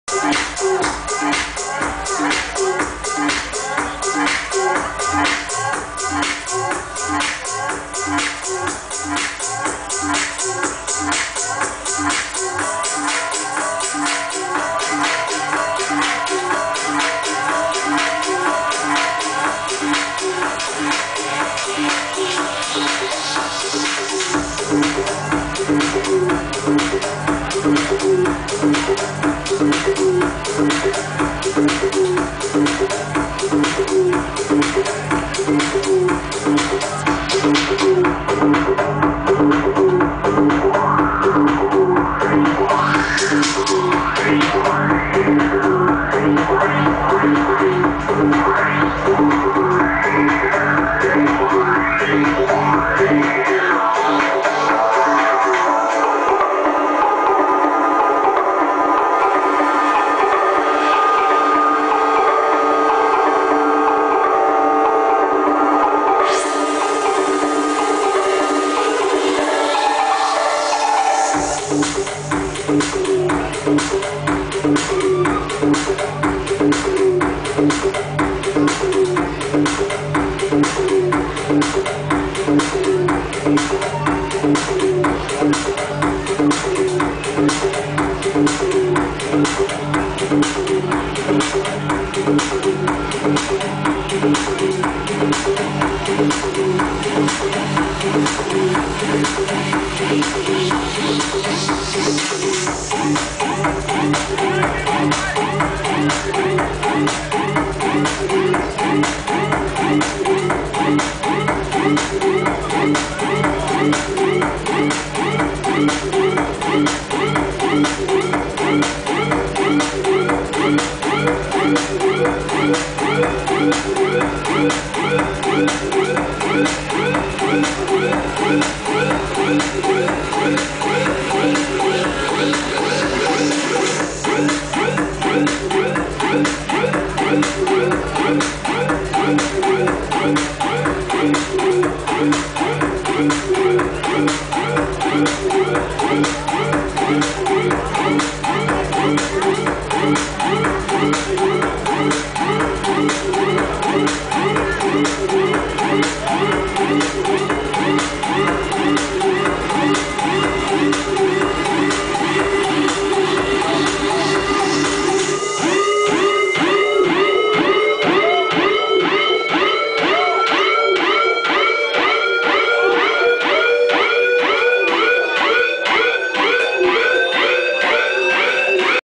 VoxHall
VoxHall er et spillested i Århus centrum, beliggende på adressen Vester Allé 15, Med plads til 500 mennesker er det hele Østjyllands nye klubscene. Her bliver præsenteret alt lige fra techno til jazz